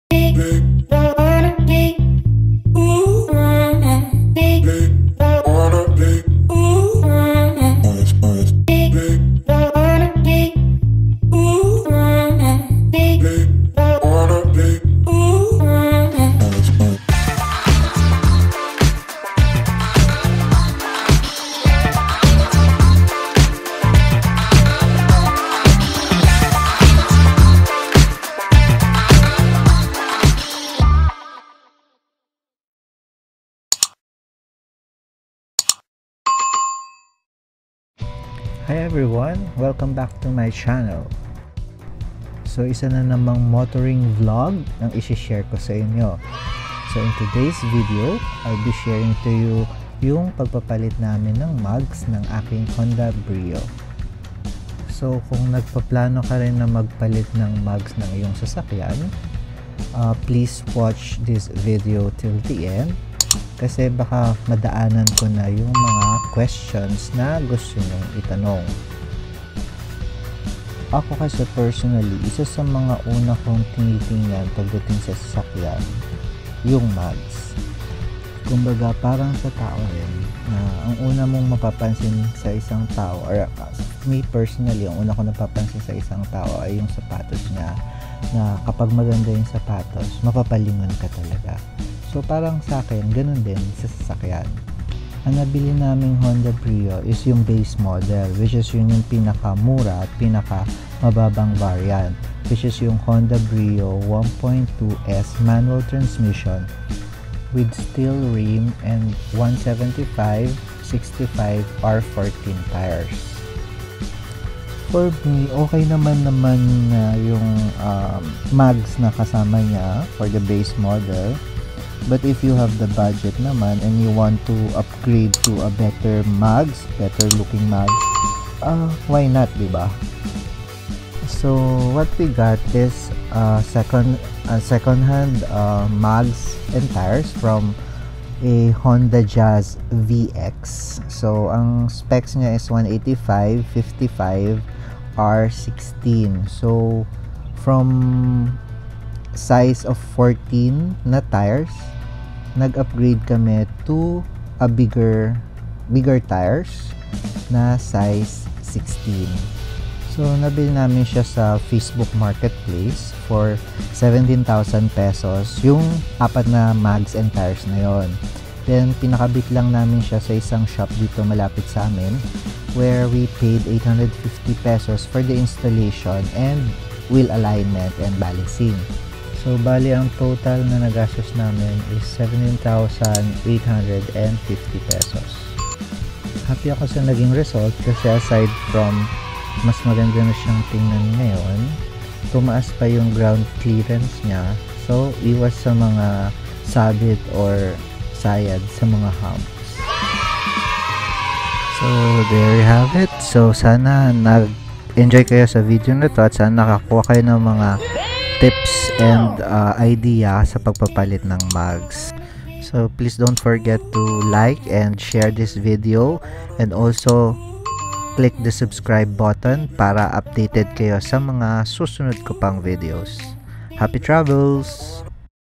MAGKANO MAGPALIT NG MAGS AT TIRES NG HONDA BRIO? Expenses, Tips and Many More!
Share ko lang yung experience ko sa pagpapalit ng mags at tires ng aking Honda Brio. If you’re planning to change your car’s tires and mags, this will be very helpful.